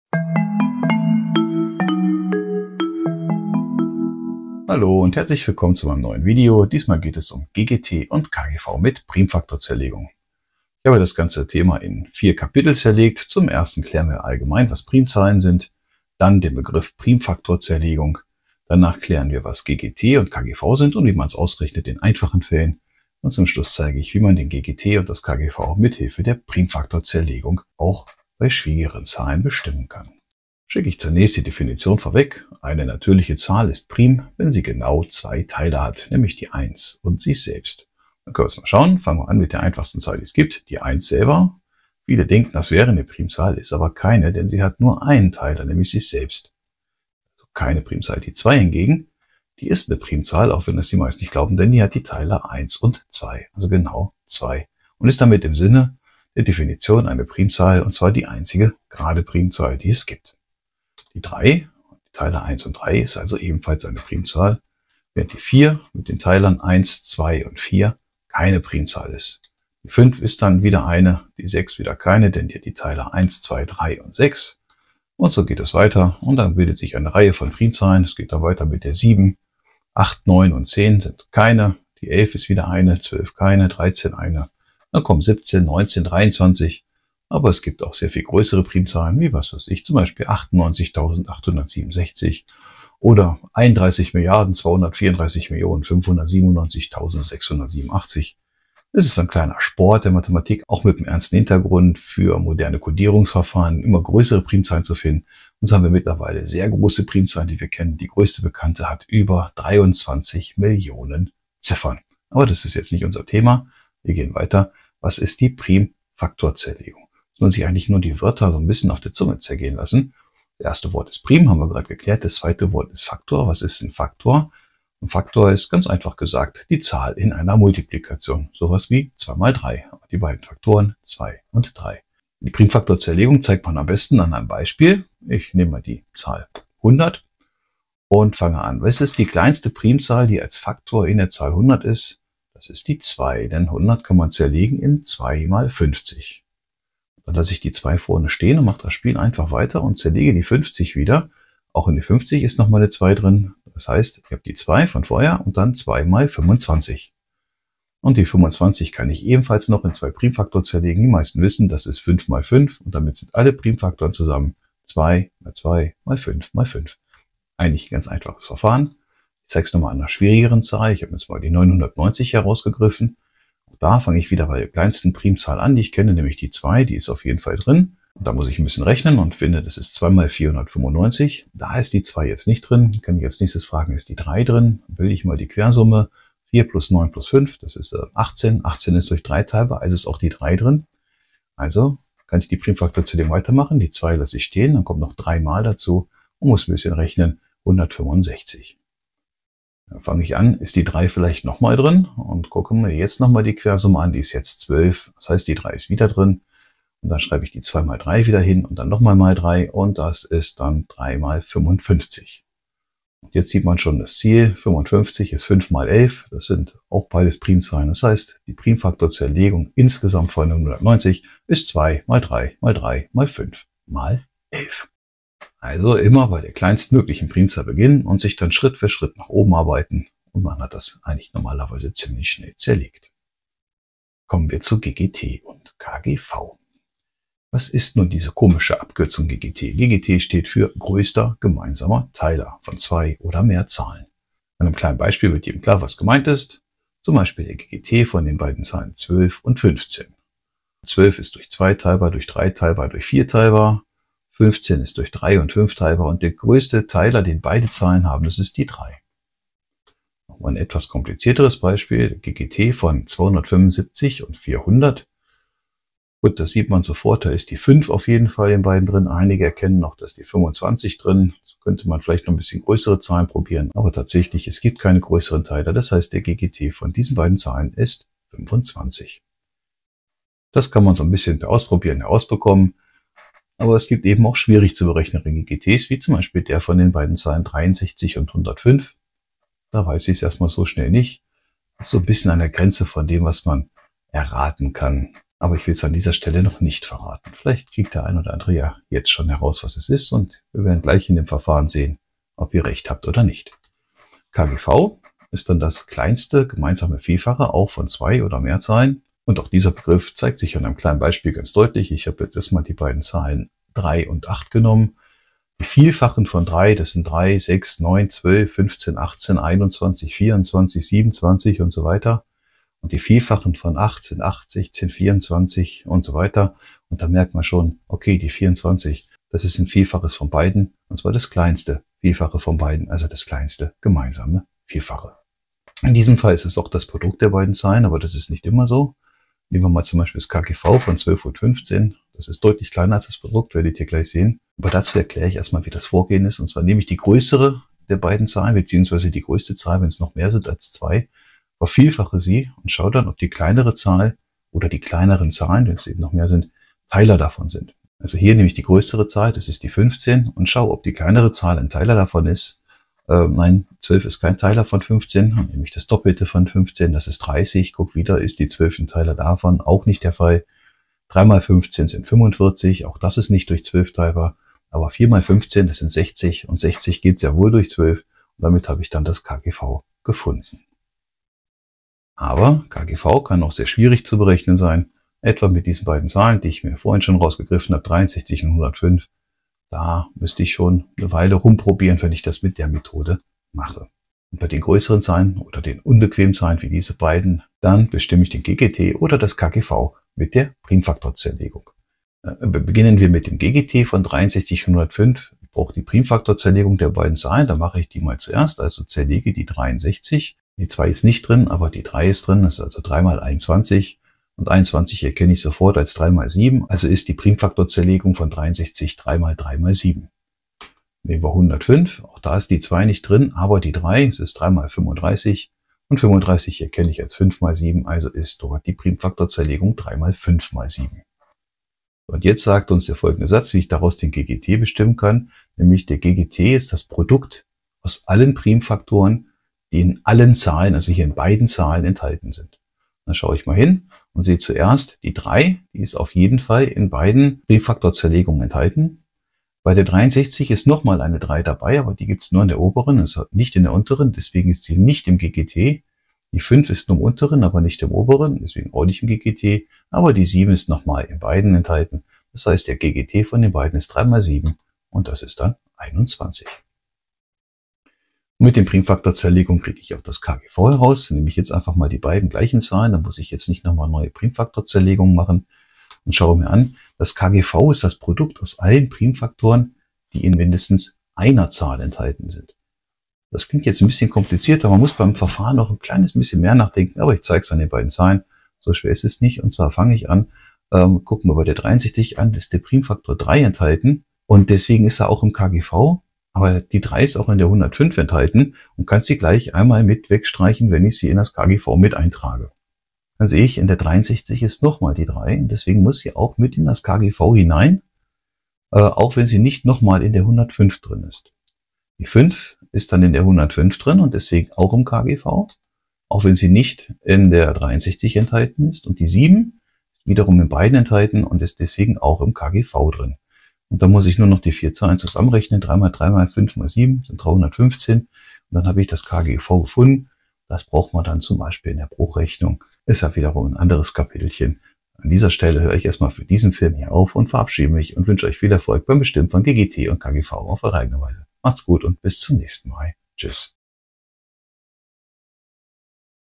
ggT und kgV mit Primfaktorzerlegung
Kapitel:
1. 0:31 Was sind Primzahlen?
2. 1:54 Was ist eine Primfaktorzerlegung?
3. 3:54 Was sind ggT und kgV und wie bestimmt man sie in einfachen Fällen?
4. 6:37 Wie bestimmt man ggT und kgV mit der Primfaktorzerlegung?

Zielgruppe:
Mathematik, Gymnasium, Sek. I, ca. 5. oder 6. Klasse